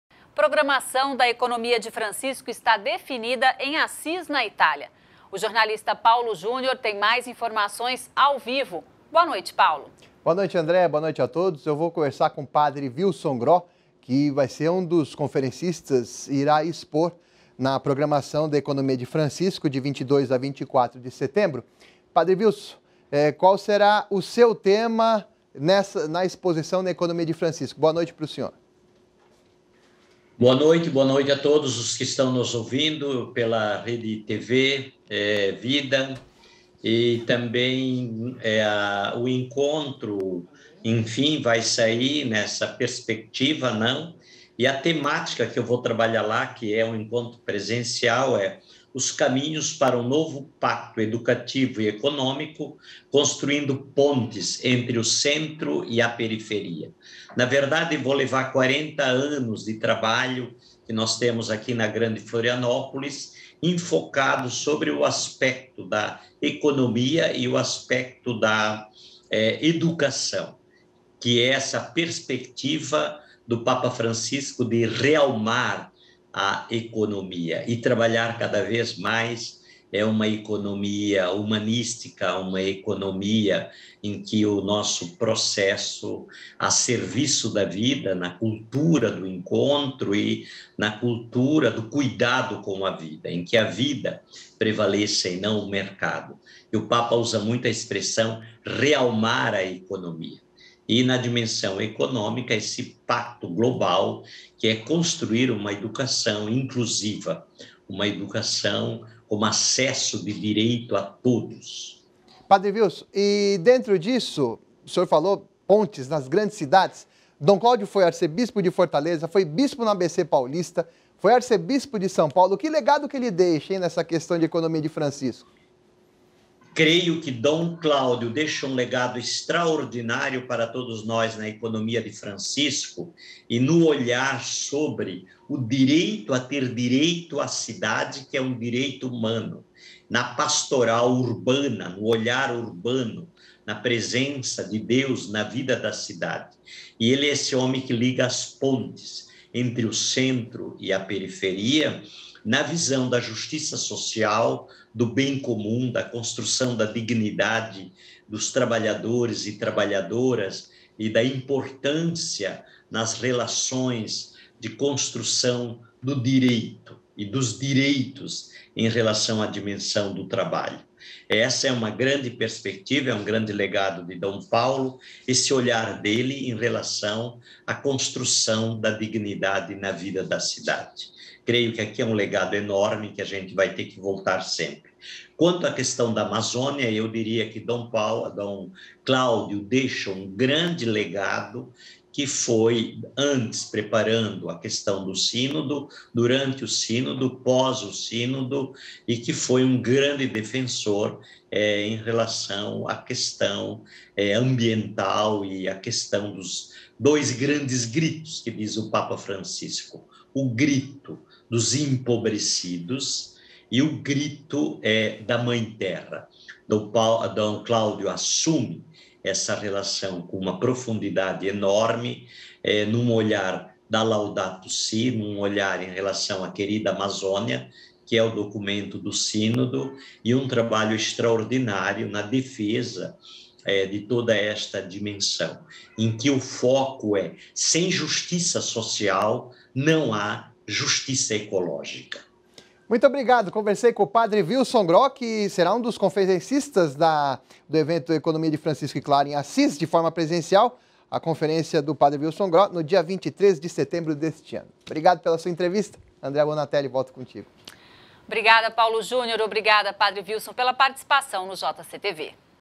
Pe. Vilson fala sua participação no semintário da Economia de Francisco.
Entre os dias 22 e 24 de setembro deste ano, Pe. Vilson Groh estará presencialmente no seminário internacional da Economia de Francisco e Clara, na cidade de Assis, na Itália.

Ele será um dos conferencistas, falando sobre “Os caminhos para o novo pacto educativo e econômico: construindo pontes entre o Centro na periferia”.

Confira a breve entrevista que Pe. Vilson concedeu recentemente ao jornal da Rede Vida e entenda um pouco do que se trata a Economia de Francisco e Clara.

Saiba mais sobre a Articulação pela Economia de Francisco e Clara (ABEFC)